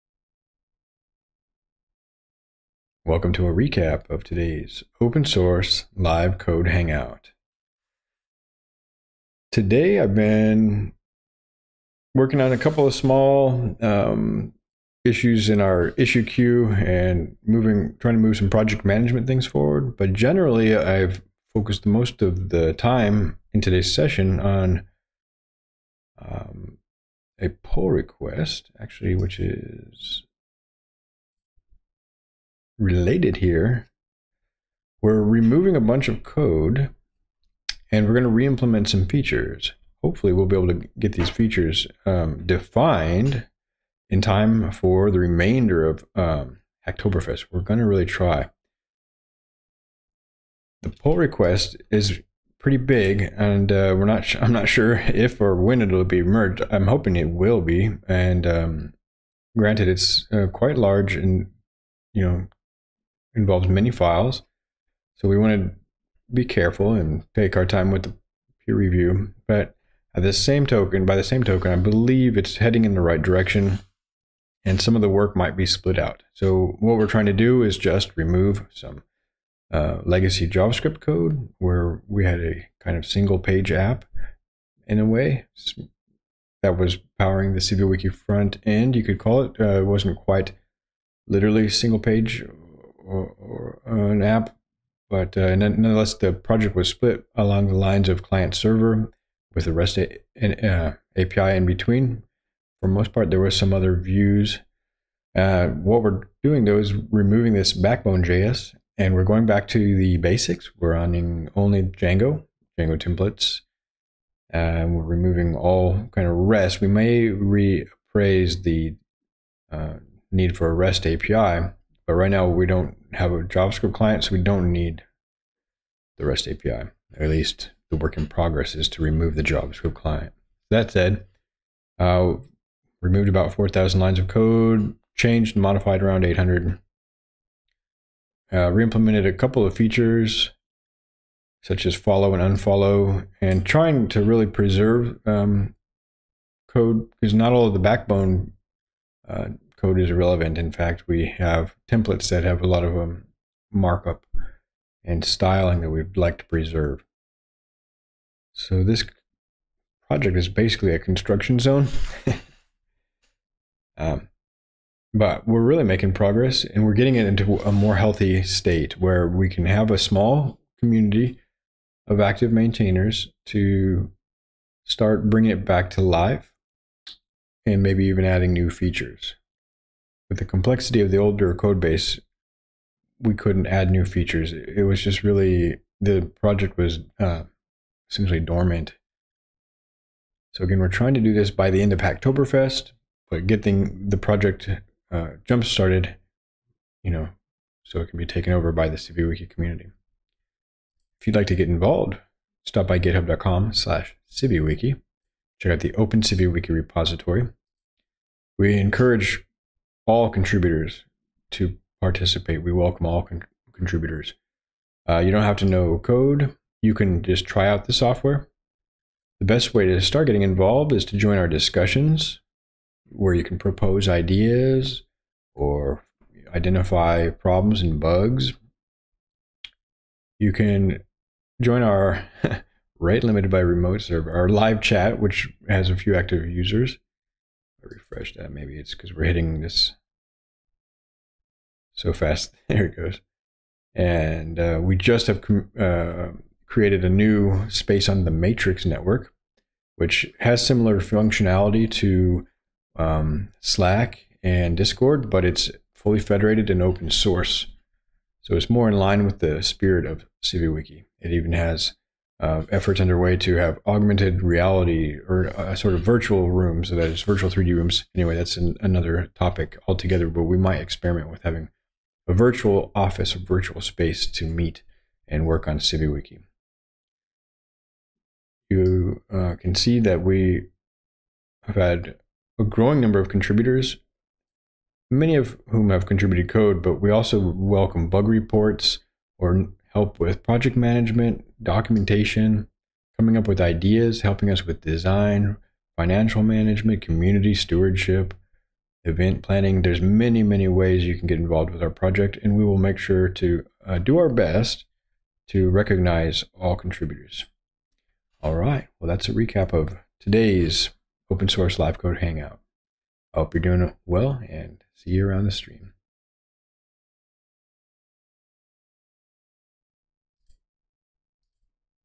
Working on a large pull request for Hacktoberfest readyness
Community stewardship for open-source projects.